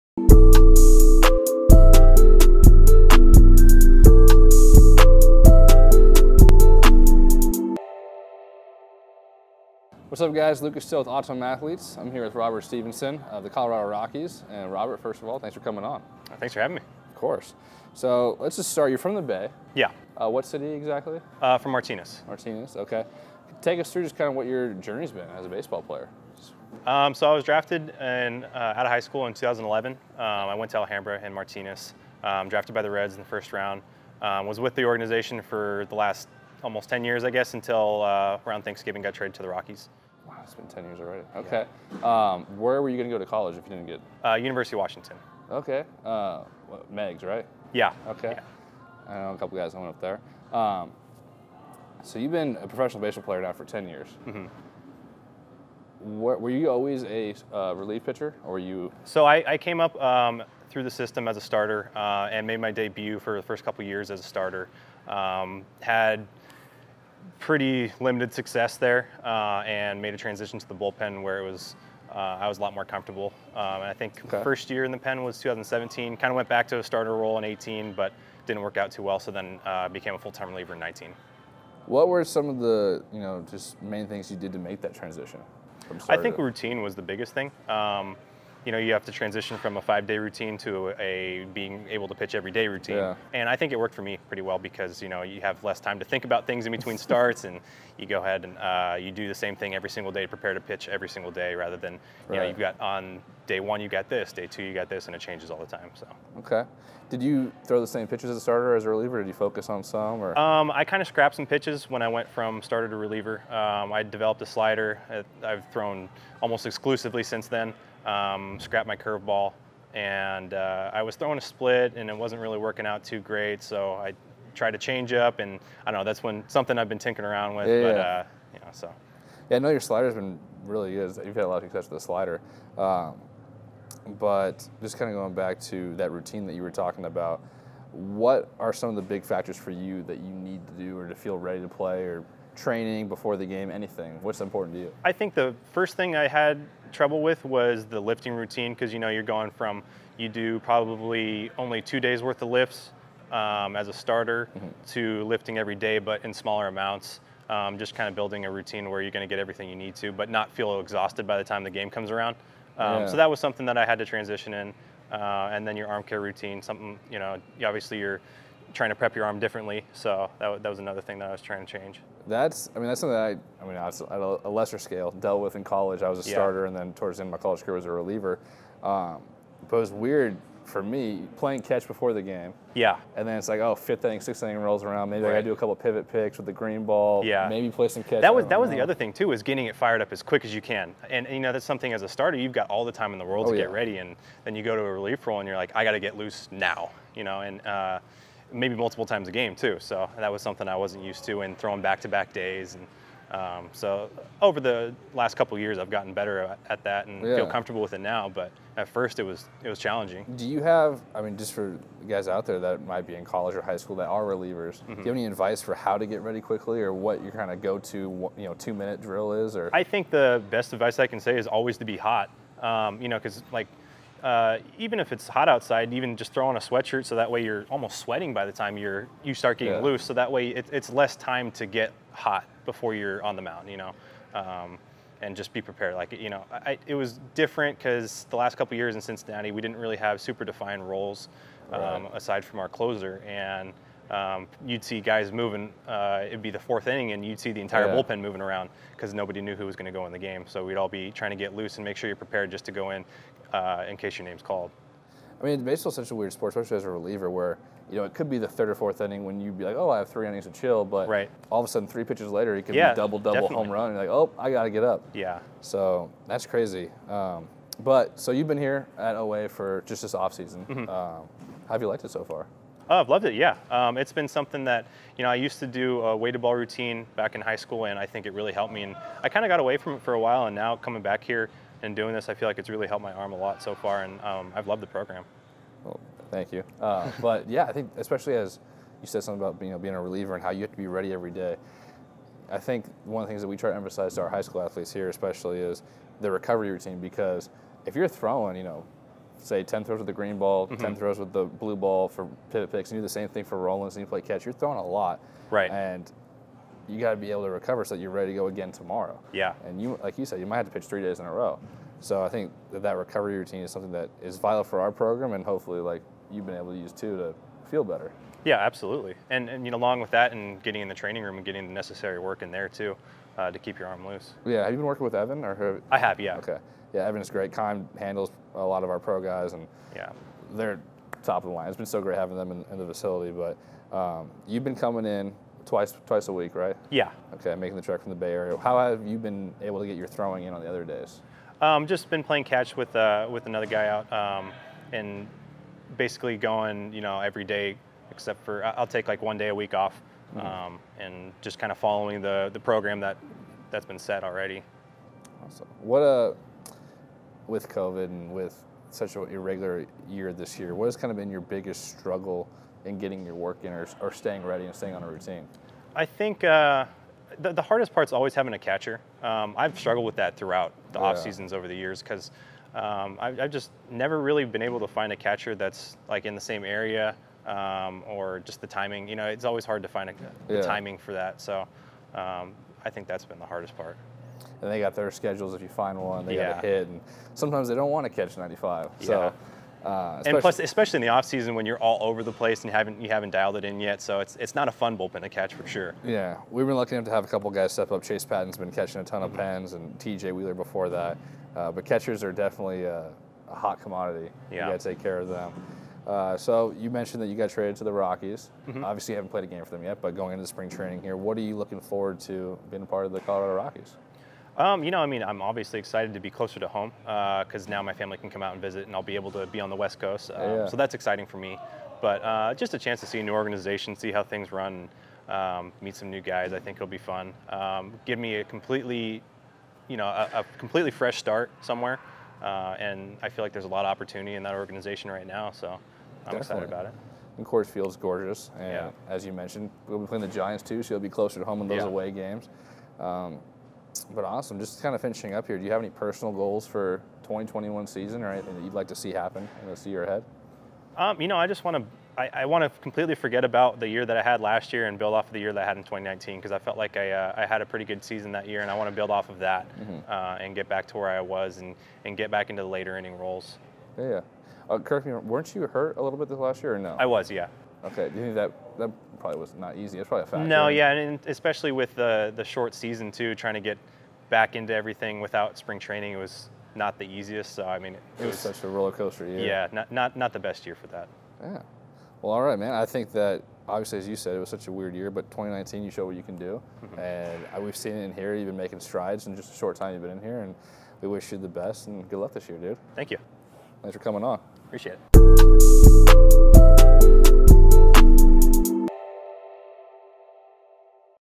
Episode 38: Robert Stephenson
Lucas sits down with Colorado Rockies reliever, Robert Stephenson, to discuss his transition to the bullpen, goals for the upcoming year, and more